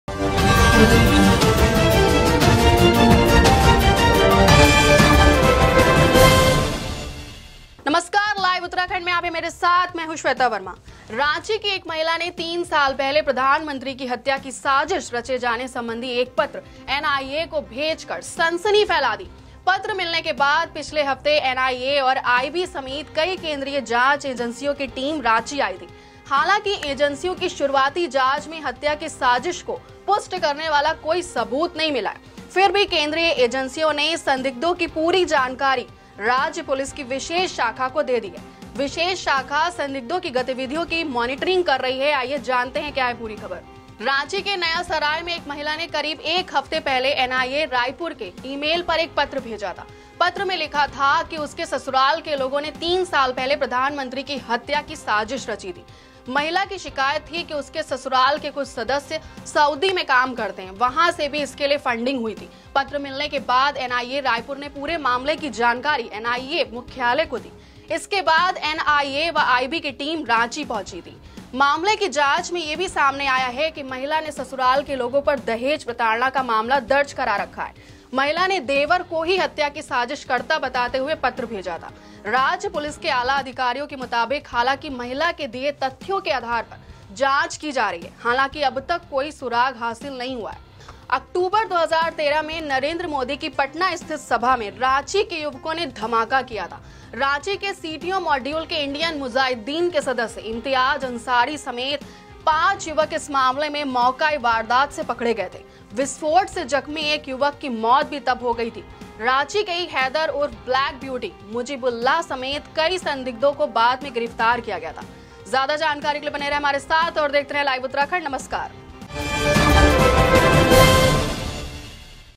PM Modi Ranchi Assassination Letter | आखिर कौन करना चाहता है PM Modi की हत्या
रांची की एक महिला ने तीन साल पहले प्रधानमंत्री की हत्या की साजिश रचे जाने संबंधी एक पत्र एनआईए को भेज कर सनसनी फैला दी। पत्र मिलने के बाद पिछले हफ्ते एनआईए और आईबी समेत कई केंद्रीय जांच एजेंसियों की टीम रांची आई थी। हालांकि एजेंसियों की शुरुआती जांच में हत्या की साजिश को पुष्ट करने वाला  कोई सबूत नहीं मिला। फिर भी केंद्रीय एजेंसियों ने संदिग्धों की पूरी जानकारी राज्य पुलिस की विशेष शाखा को दे दी है। विशेष शाखा संदिग्धों की गतिविधियों की मॉनिटरिंग कर रही है।